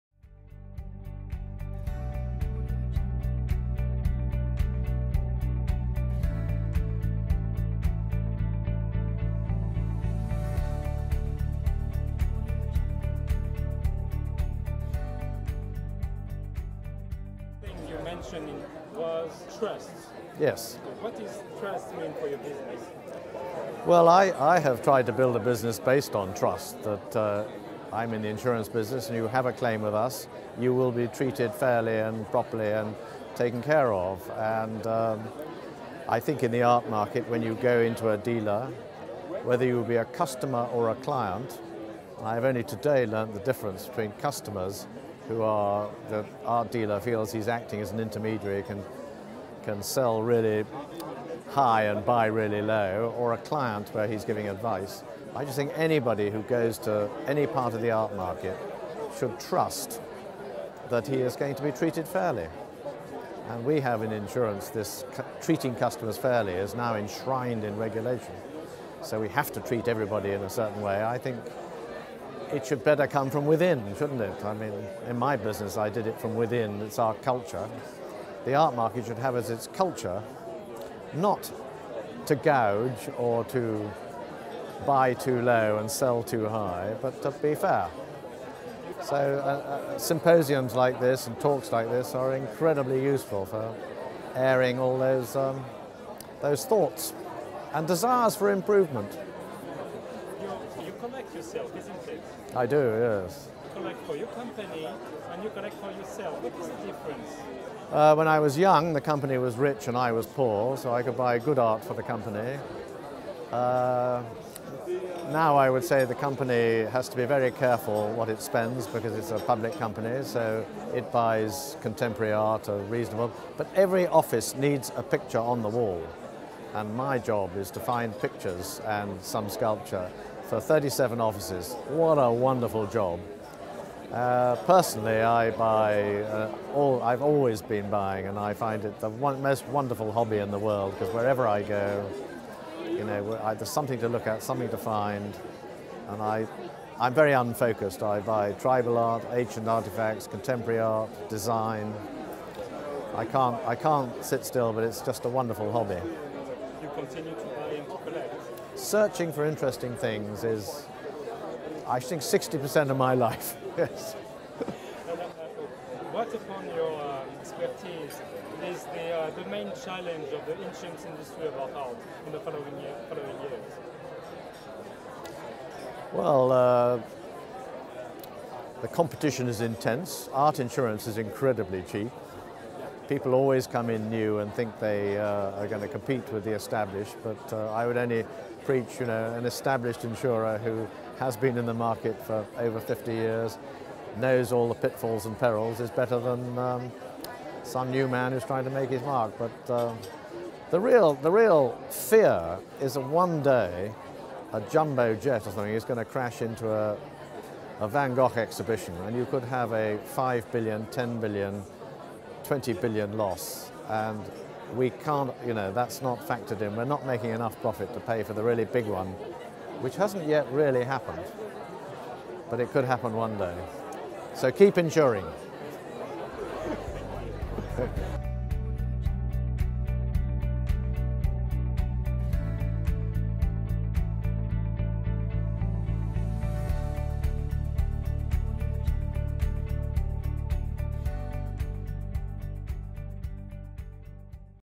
Art Business Conference 2015 : A talk with Robert Hiscox
A short talk with Robert Hiscox just after his attendance as a speaker at the Art Business Conference.

Robert Hiscox  has spent the last 50 years helping to build the Hiscox insurance business and buying art for himself and the company.   He has a wealth of experience of the foibles of the art trade and the perils faced by both collectors and the works of art.

a BIAPAL Production